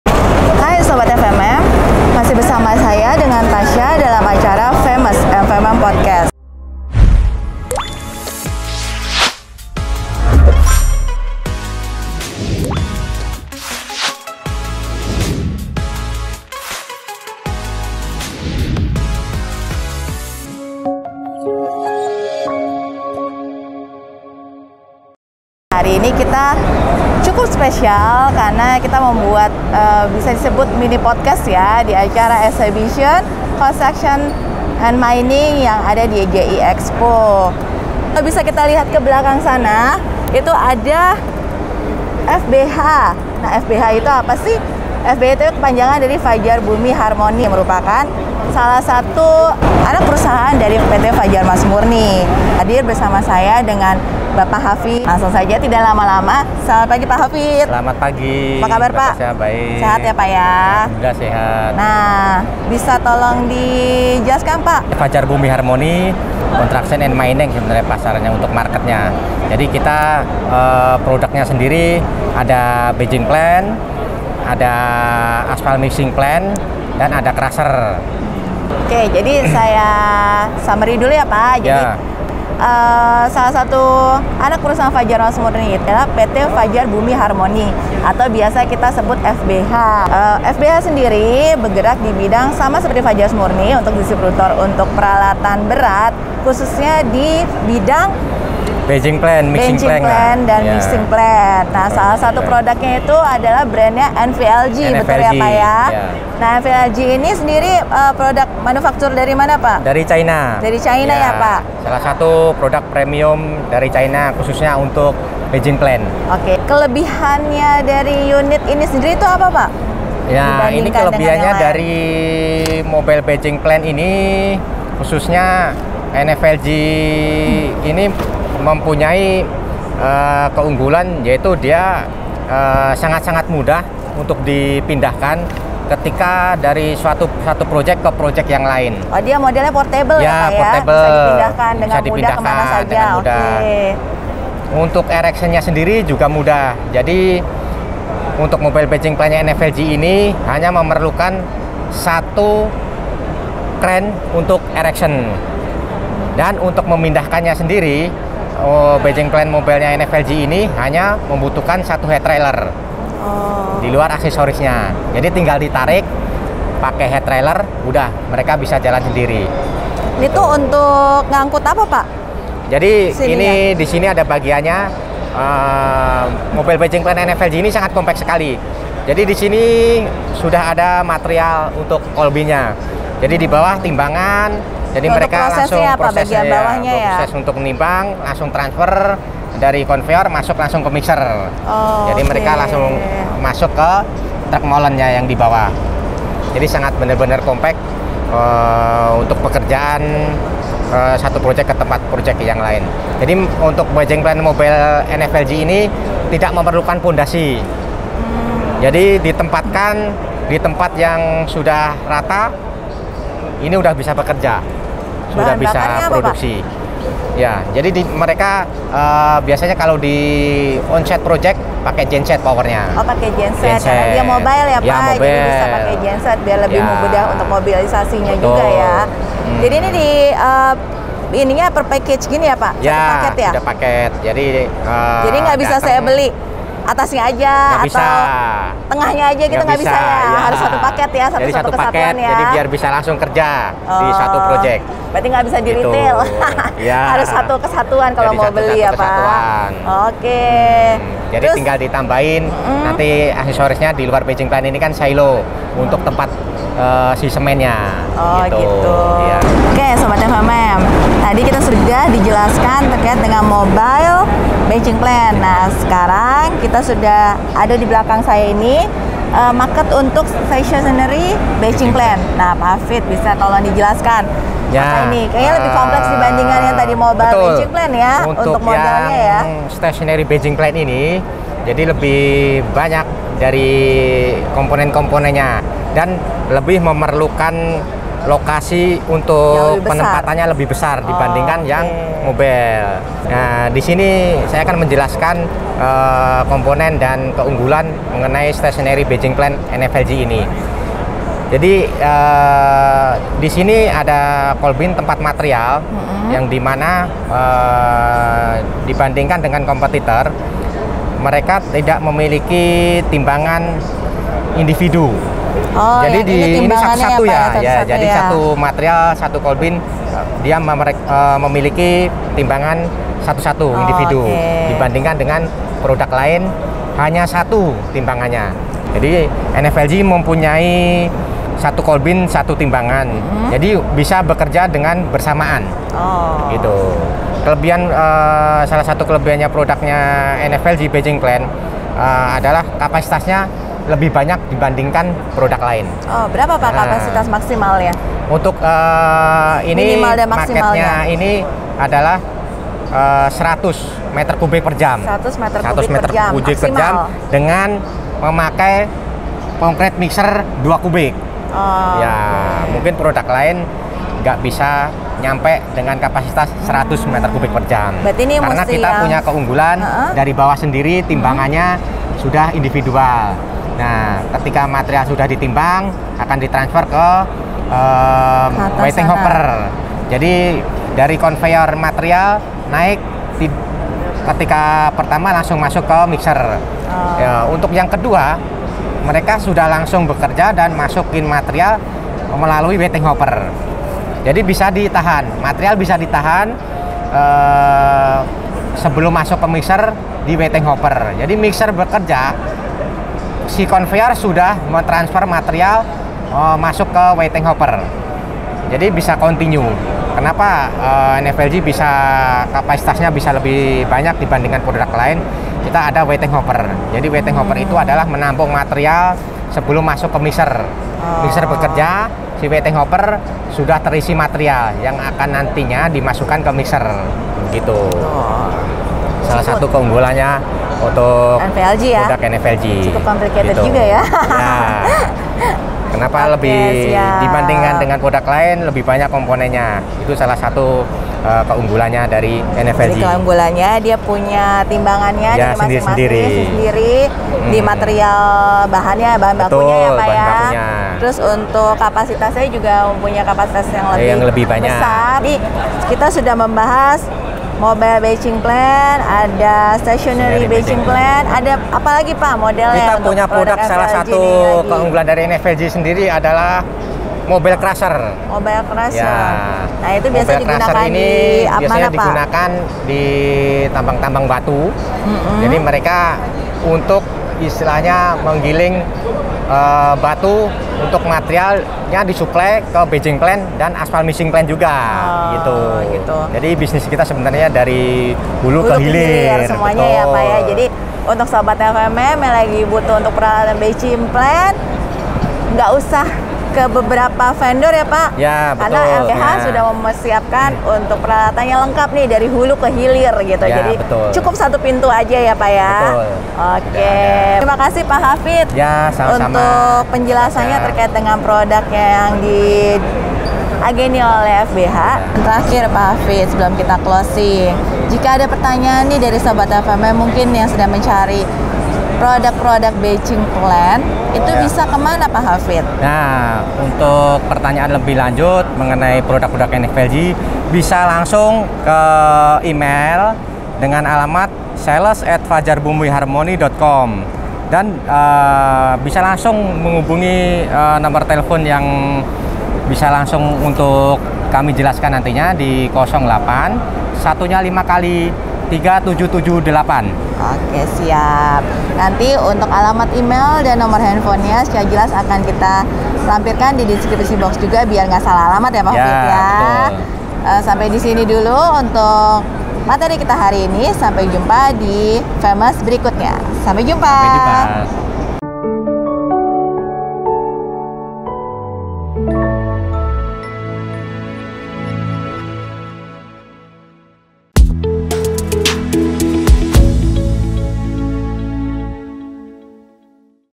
Mesin Batching Plant Handal
Hai...sobatfmm sedang mencari peralatan batching plant??
Materi FMMOUS kita hari ini akan membahasa jenis, keunggulan peralatan batching plant yang diageni oleh salah satu anak perusahan PT Fajar Mas Murni yaitu PT Fajar Bumi Harmoni...

Penasaran harga dan keunggulan produk apa saja?? langsung saja di simak sampai habis...

Jangan sungkan menghubungi tim kami untuk perlengkapan batching plant anda

Cp. Bpk Havid